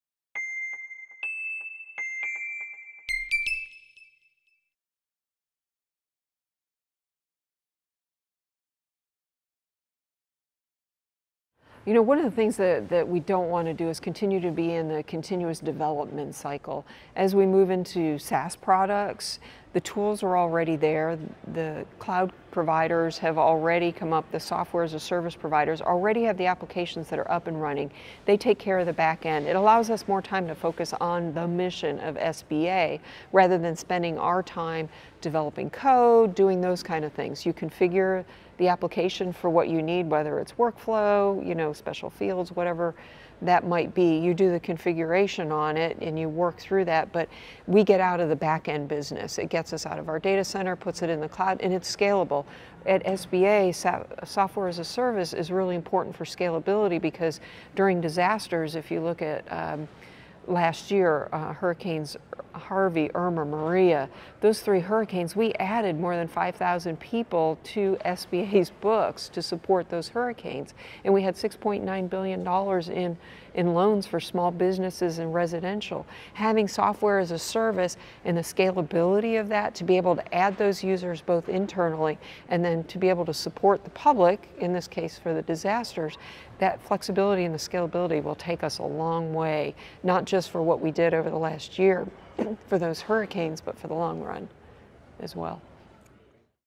Trailblazers in Government: SBA's Maria Roat, Part 2
Answering the question: Why did you or your team believe that SaaS cloud models offered the best solutions for what your agency is trying to accomplish?